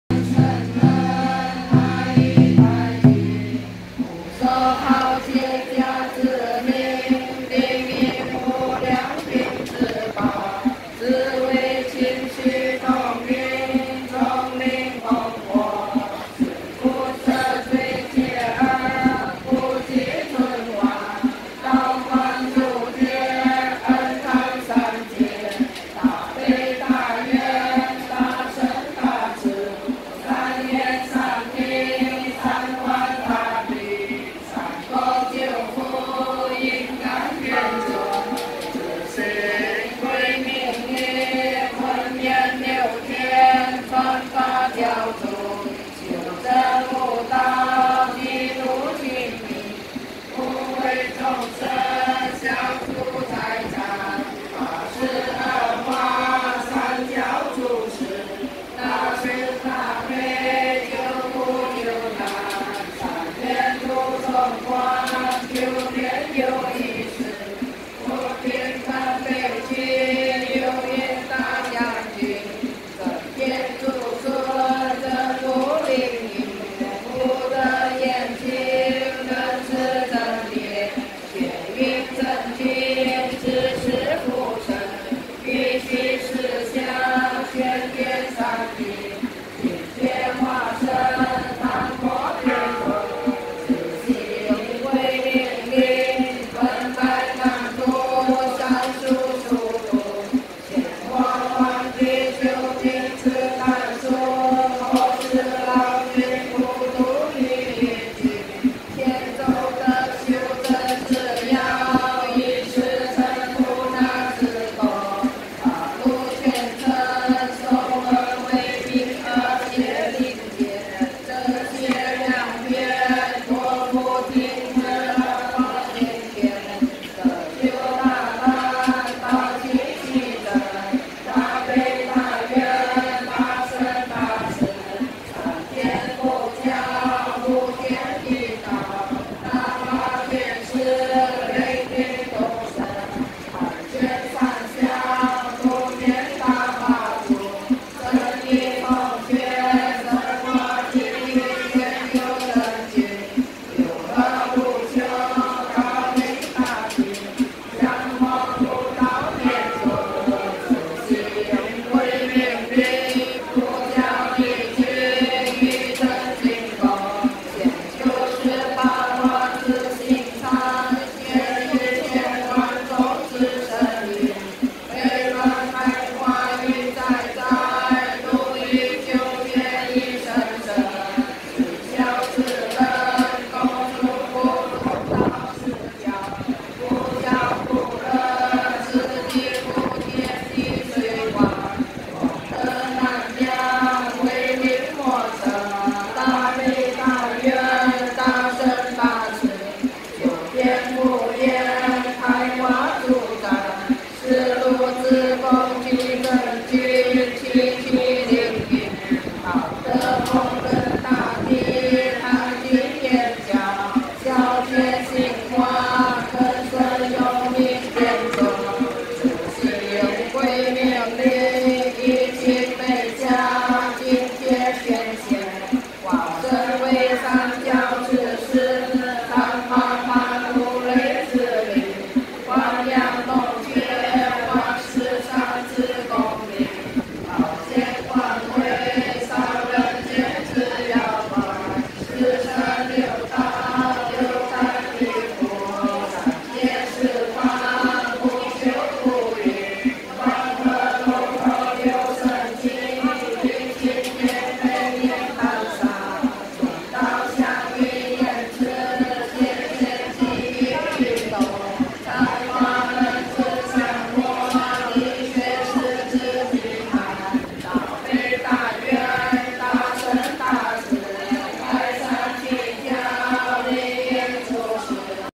正龍宫玄江殿 - 冥阳超宗普渡 2018 (9)
Day 2 of 3 - rituals for salvation of the spirits.